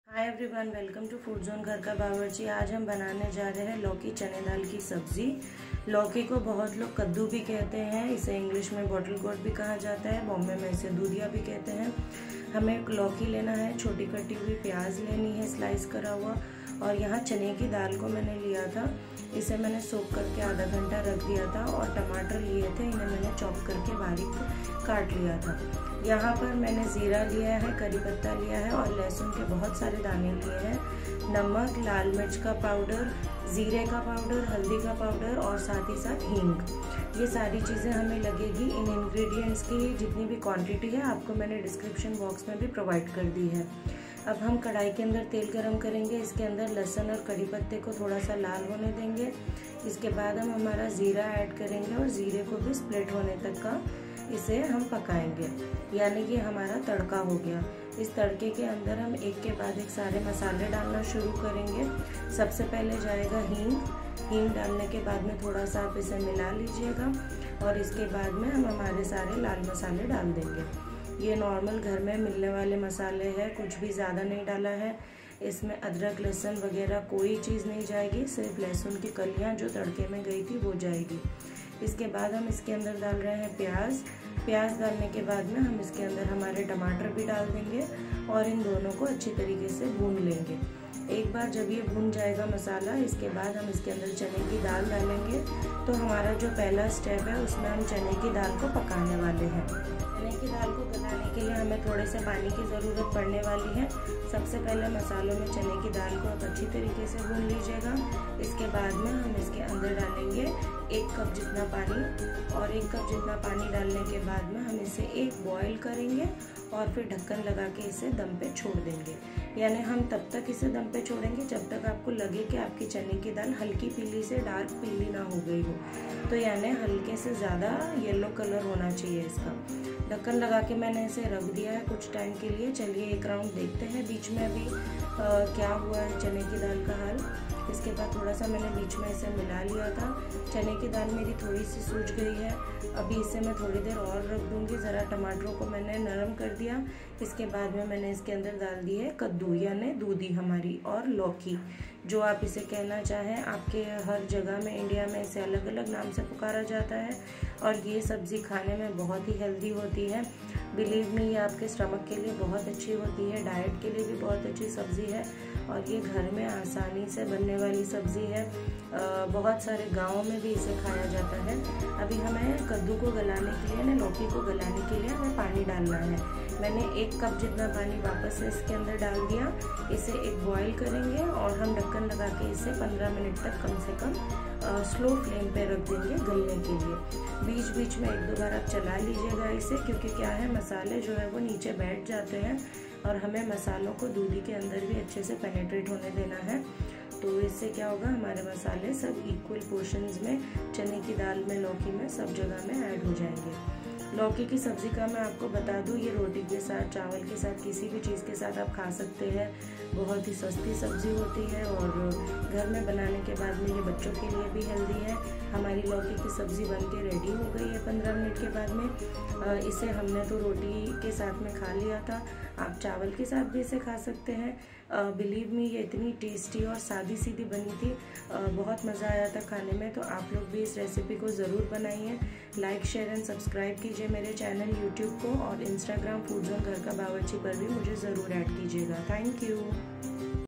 Lauki Chana Dal Sabzi | Bottle gourd Bengal Gram Curry | Indian simple Veg gravy | Dudi Kaddu Sabji
ENJOY THIS SIMPLE YET HEALTHY VEGETABLE CURRY.

🔔❤️S.Irani

🌍foodzone_gharkabawarchi

(SERVES - 4-5 persons)

INGREDIENTS

Bottle gourd / लौकी - 1 kg

Tomatoes / टमाटर - 2

Onions / प्याज़ - 3 sliced

Bengal Gram / चने की दाल - 100gms

Curry leaves / करी पत्ता - 8-10

Garlic Cloves / लहसून - 15-20

Cumin seeds /  जीरा - 1 tbspn

SPICES

Red chilli powder / लाल मिर्च- 1 tbsp

Turmeric powder / हल्दी- 1 tspn

Salt / नमक - 1 tbspn

Cumin powder / जीरा - 1 tspn